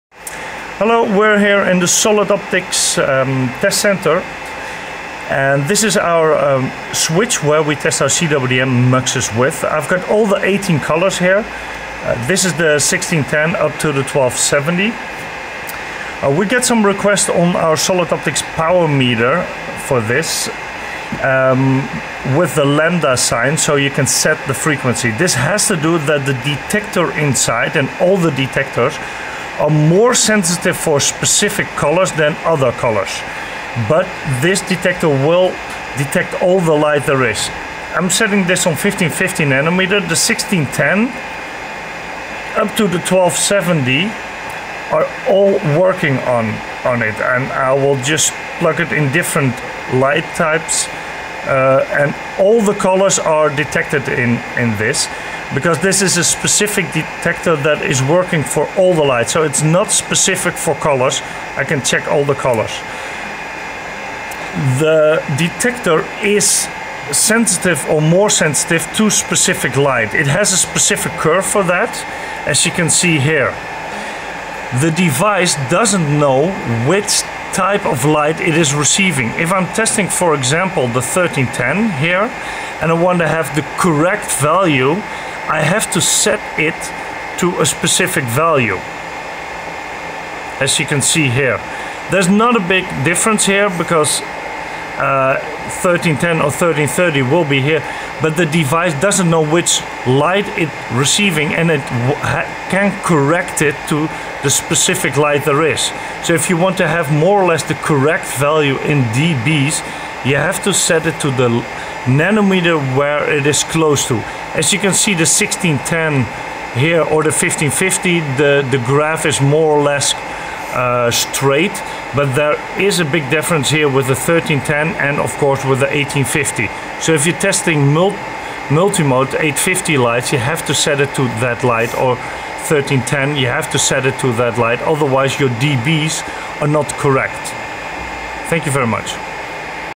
CWDM / DWDM Tutorial 04 - Optical-Power-Meter Lambda settings
This Movie explains how to use the Lambda button on the Solid Optics Optical Power Meter. The Power Meter uses an InGaAs detector which has unique sensitivity for different wavelengths. To calibrate the correct dBm, the Power Meter must be set to a specific wavelength which is close to the wavelength you are measuring.